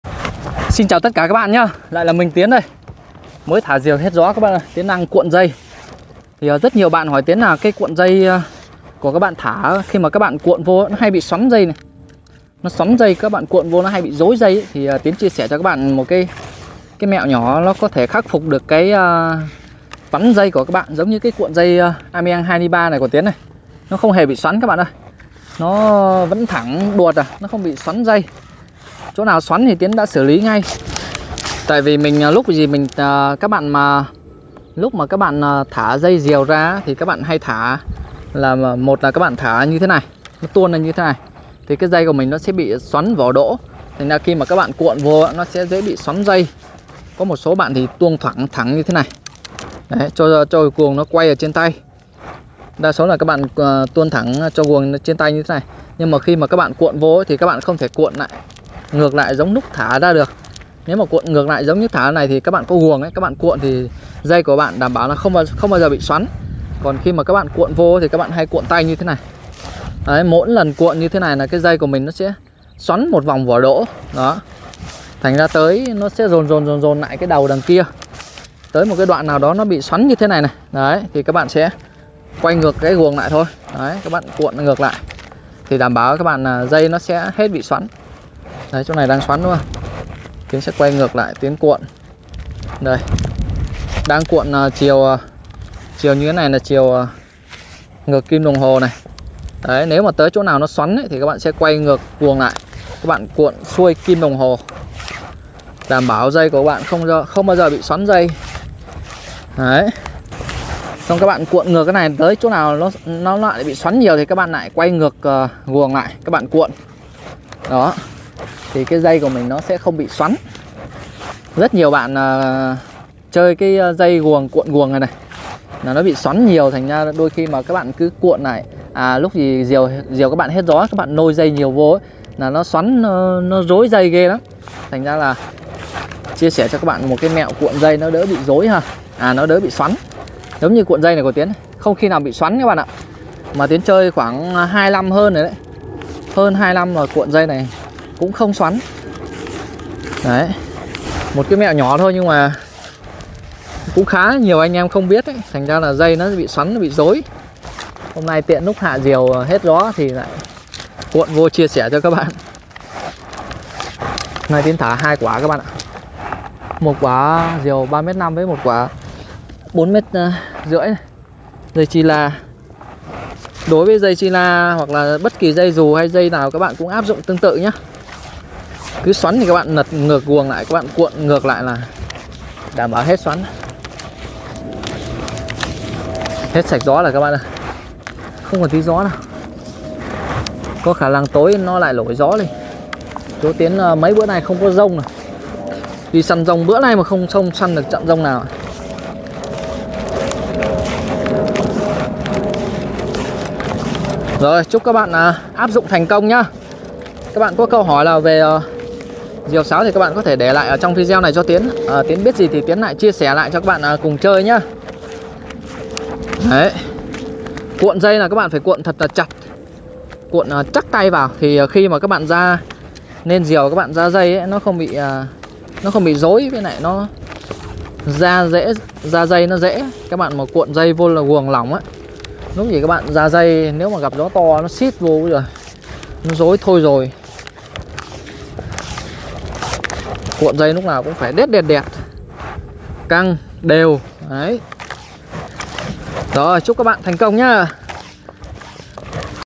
Tiến BVT | Cách Khắc Phục Dây Thả Diều Sáo Bị Xoắn
Dây thả diều sáo sau một thời gian thả sẽ bị xoắn các bạn tham khảo cách khắc phục nha, hôm nay đi thả diều sáo mà gió hơi bé các bạn ơi cuộn dây thả diều sáo lại tiện chia sẻ cho các bạn .
 Các bạn có câu hỏi nào về diều sáo cứ để lại cho mình nha.
BUI VAN TIEN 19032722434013 Techcombank.
Cảm ơn tấm lòng yêu qúy của các bạn dành cho Tiến rất nhiều.